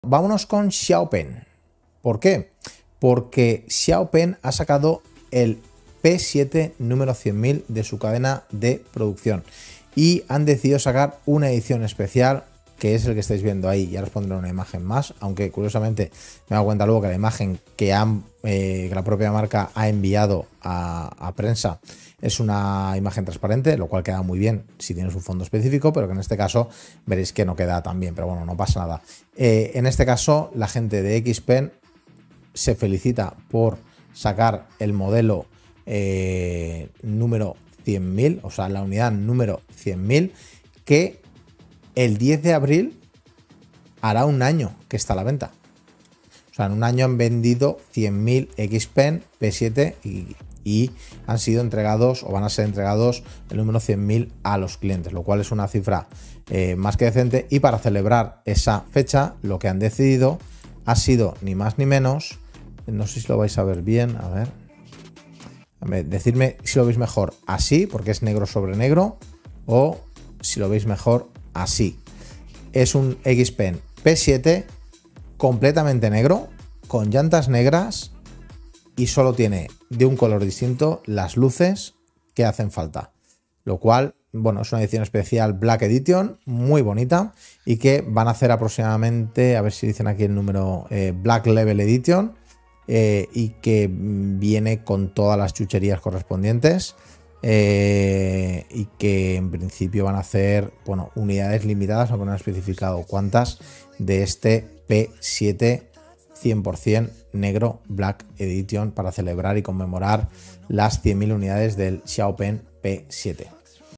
Xpeng P7: 100.000 unidades y EDICIÓN Especial!
Desde Xpeng están de celebración porque tenemos la unidad 100.000 del P7 saliendo de la línea de producción por lo que Xpeng sacó una edición limitada con un acabado 100% black Edition.